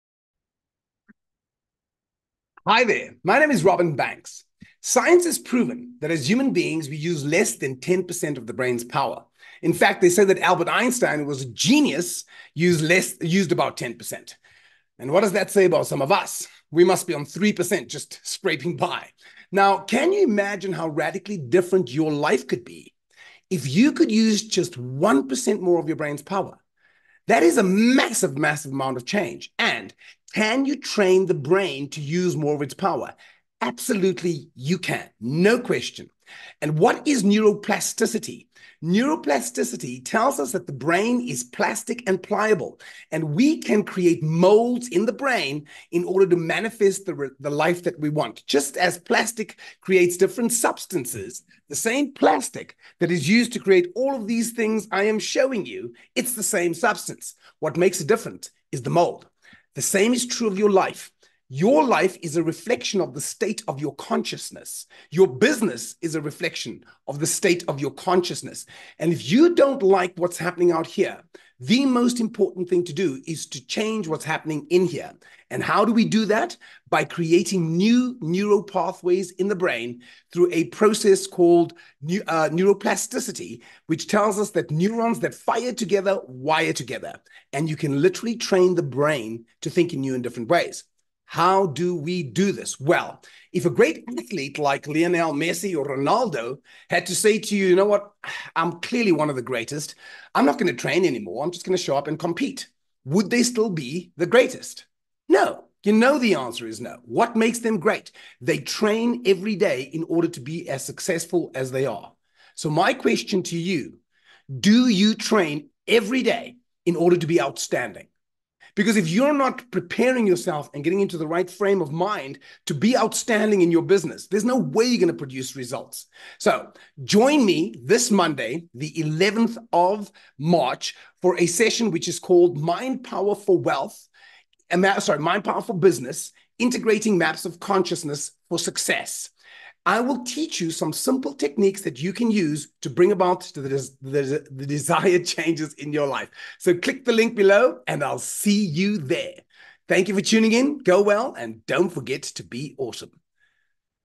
Mind Power for Business
The secret to a great life - and business:

I have the absolute pleasure to invite you to a dedicated master class next Monday, March 11, 5 pm CET😃

Robin Banks is hosting this for my community and he will show you how to unlock your full potential with neuroplasticity 💥💣😃